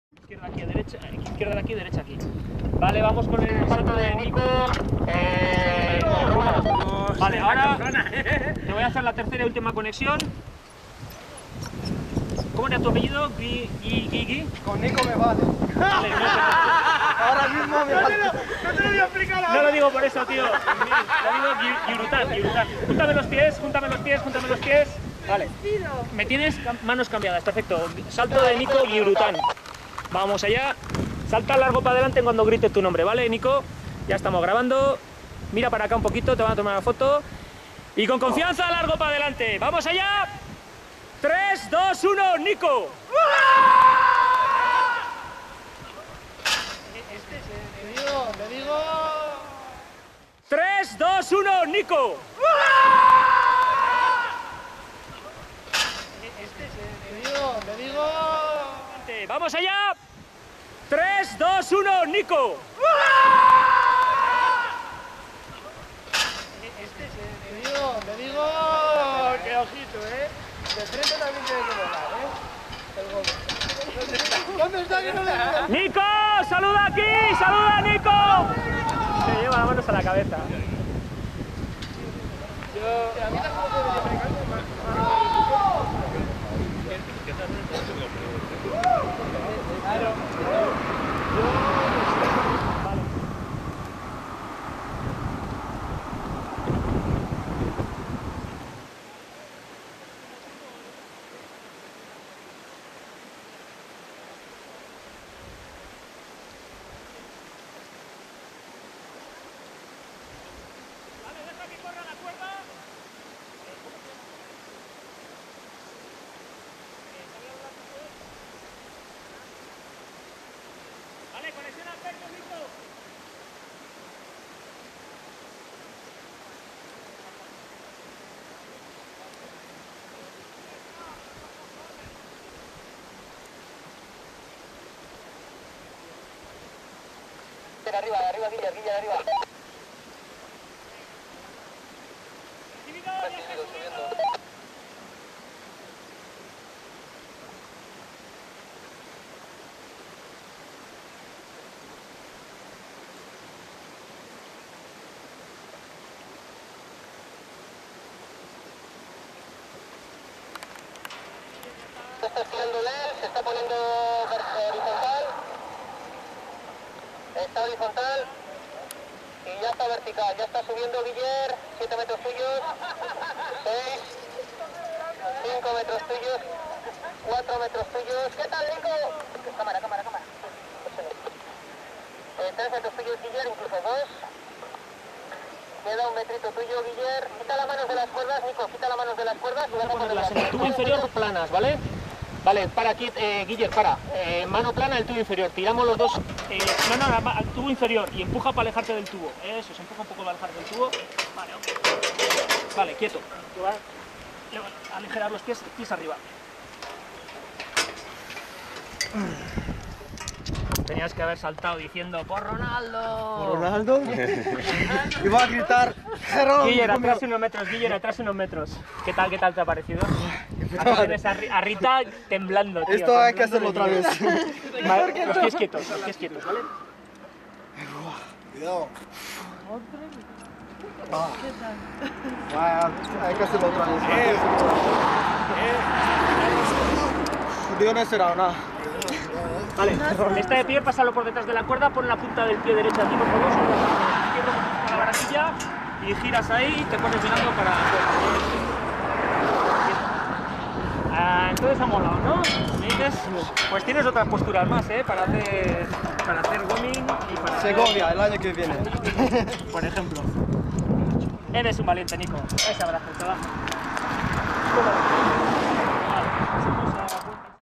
Góming Cofio NBG de cabeza y de frente 3C.mpg
Era el primer salto de Nico. Y tenía ganas de experimentarlo. Auque una vez al borde del puente las sensaciones no eran las que se esperaban o se habían pensado de antemano (soltar una frase como "Con Nico me vale" en ese momento resulta muy significativo). De todos modos, la cuestión es echarle valentía, y fiarse. Nico lo hizo.